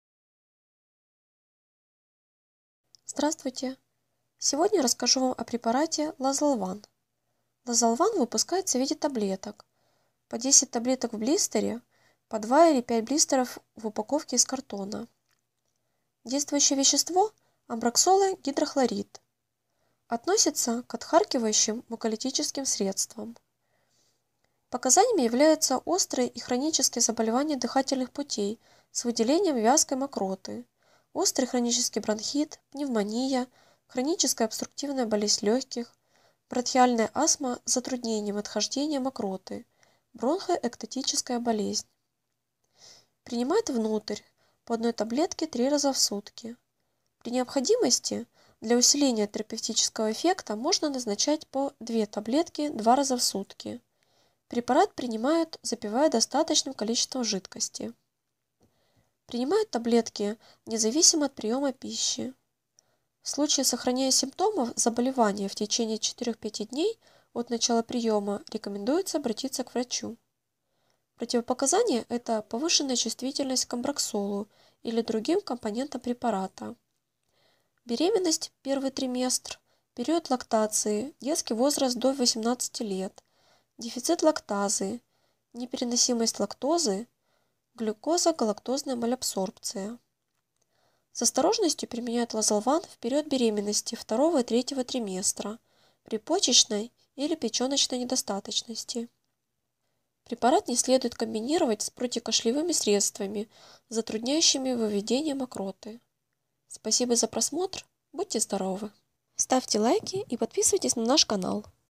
Лазолван таблетки - показания (видео инструкция) описание, отзывы - Амброксола гидрохлорид
Лазолван таблетки – отхаркивающее и разжижающее мокроту лекарство
B исследованиях показано, что активный ингредиент данного лекарства (амброксол) увеличивает секрецию в респираторных (дыхательных) путях. Он способствует нормализации секреции серозных клеток желез, находящихся в слизистой бронхов, разжижает вязкий бронхиальный секрет, значительно облегчает его отхождения за счет улучшения мукоцилиарного клиренса (продвижения слизи реснитчатыми клетками). Действуя как муколитическое (разжижающее) средство и экспекторант (способствует быстрому выведению мокроты из бронхов), улучшает показатели внешнего дыхания. Кроме того, он повышает количество сурфактанта (поверхностно-активного вещества, выстилающего альвеолы изнутри и препятствующего спадению альвеол), увеличивая его продукцию в клетках легких, а также препятствуя его распаду. B исследованиях длительное лечение препаратом (на протяжении не менее двух месяцев) приводило к снижению числа обострений y пациентов c хронической обструктивной болезнью легких. Отмечалось также достоверное уменьшение длительности обострений, числа дней антибиотикотерапии.

Нельзя назначать раствор Лазолван при аллергии на амброксол, другие компоненты лекарства, беременности (в I-м триместре), лактации.

Внимание!